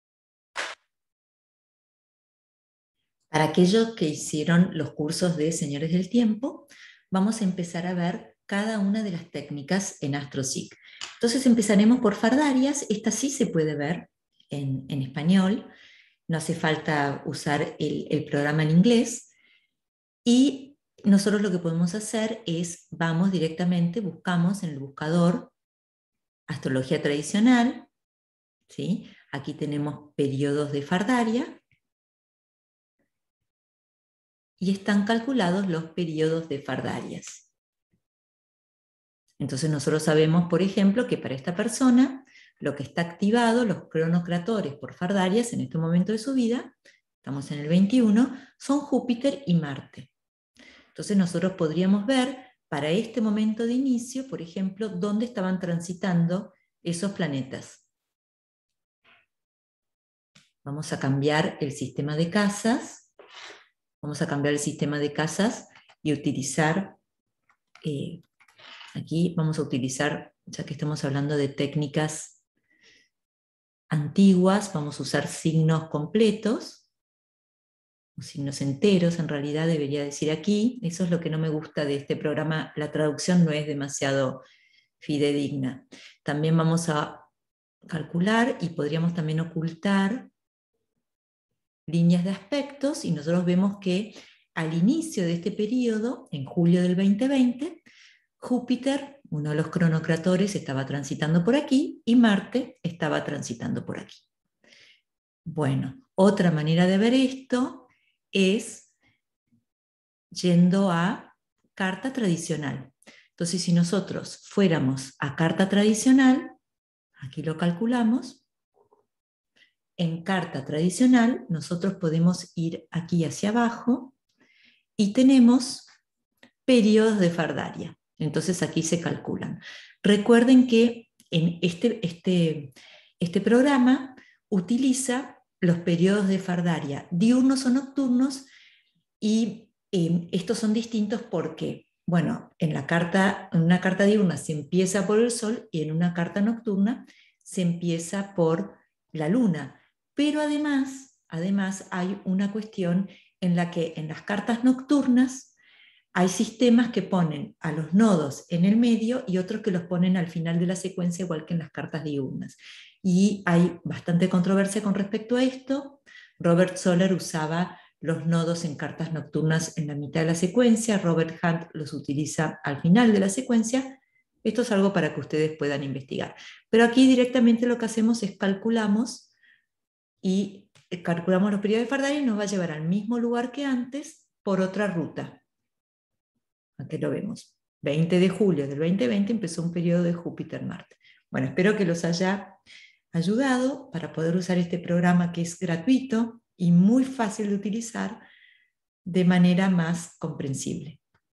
Fardarias en Astroseek
Astrología Tradicional  Fardarias  Señores del Tiempo en Astroseek

Como levantar las Fardarias en Astroseek

Para aprender más acerca de los cursos de la Academia, ingresa a nuestra página

Link a mi libro